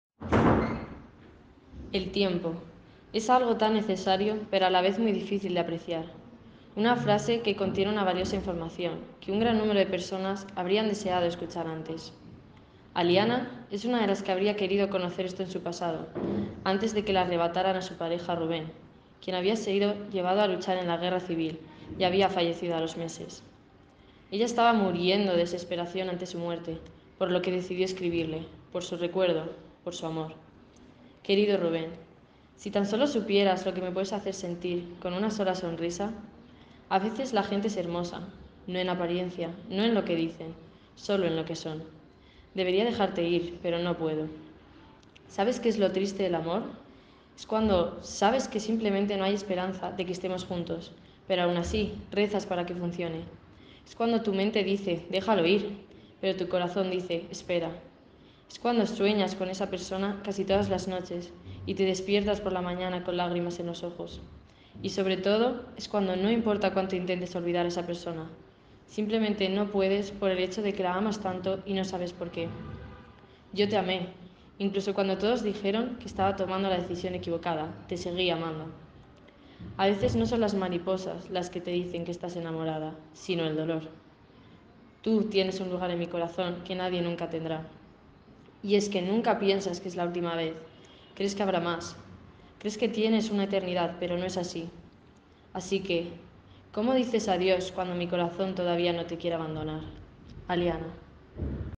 Colegio Arturo Soria | 4º ESO | 4ºA (Grupo 1) | Jimena Talavera Hoyos
Centro Educativo:
Colegio Arturo Soria
Curso:
4º ESO
Aula:
4ºA (Grupo 1)
Nombre del Alumno/a:
Jimena Talavera Hoyos
Nombre del Profesor/a:
Sandra Muñoz Herrero

Microrrelato:
"El tiempo, es algo tan necesario pero a la vez muy difícil de apreciar. Una frase que contiene una valiosa información que un gran número de personas habrían deseado escuchar antes.

Aliana es una de las que habría querido conocer esto en su pasado, antes de que le arrebataran a su pareja, Rubén, quien había sido llevado a luchar en la guerra civil y había fallecido a los meses. Ella estaba muriendo de desesperación ante su muerte por lo que decidió escribirle, por su recuerdo, por su amor.

Querido Rubén:
Si tan solo supieras lo que me puedes hacer sentir con una sola sonrisa. A veces la gente es hermosa. No en apariencia. No en lo que dicen. Sólo en lo que son. Debería dejarte ir pero no puedo.
¿Sabes qué es lo triste del amor? Es cuando sabes que simplemente no hay esperanza de que estemos juntos, pero aún así rezas para que funcione. Es cuando tu mente dice ‘déjalo ir’ pero tu corazón dice ‘espera’. Es cuando sueñas con esa persona casi todas las noches y te despiertas por la mañana con lágrimas en los ojos. Y sobre todo, es cuando no importa cuánto intentes olvidar a esa persona, simplemente no puedes por el hecho de que la amas tanto y no sabes por qué.
Yo te amé, incluso cuando todos dijeron que estaba tomando la decisión equivocada te seguí amando. A veces no son las mariposas las que te dicen que estás enamorada, sino el dolor. Tú tienes un lugar en mi corazón que nadie nunca tendrá.
Y es que nunca piensas que es la última vez, crees que habrá más. Crees que tienes una eternidad pero no es así. Así que, ¿cómo dices adiós cuando mi corazón todavía no te quiere abandonar?

Aliana."

Obra de referencia:
Mala Luna, de Rosa Huertas